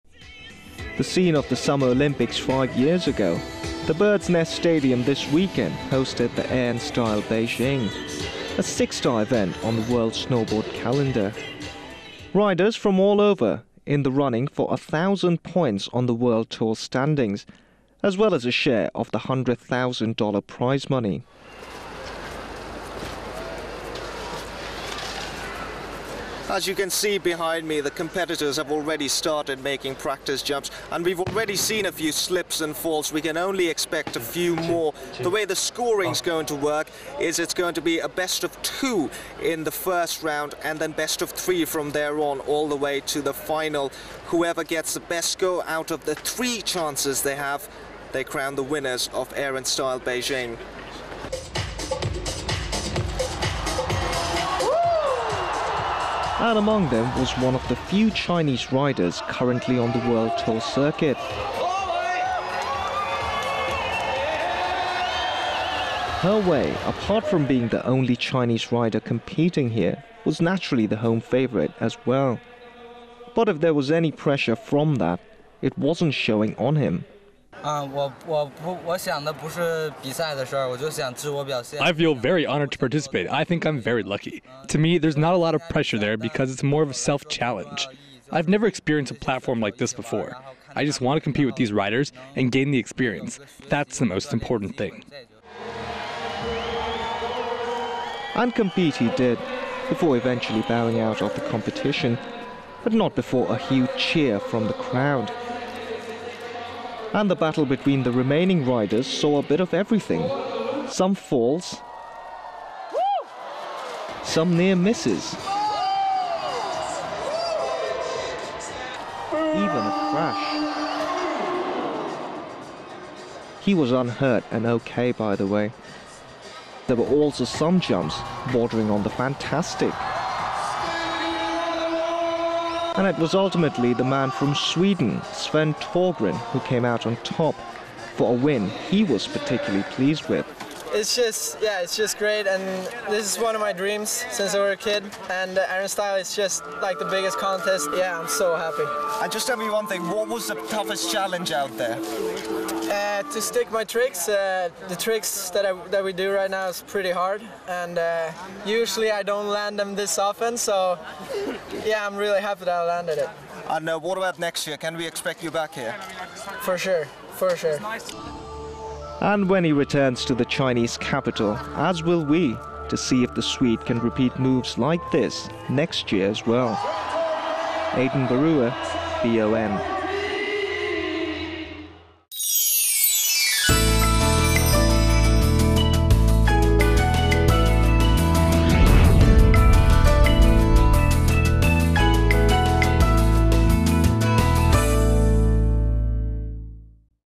Snowboarding Air & Style Beijing 2013 - Blue Ocean Network (BON)
A 6-star event on the World Snowboard Tour comes to the Beijing Olympic Stadium. Riders compete for 1000 points on World Tour standings and $100,000 prize money.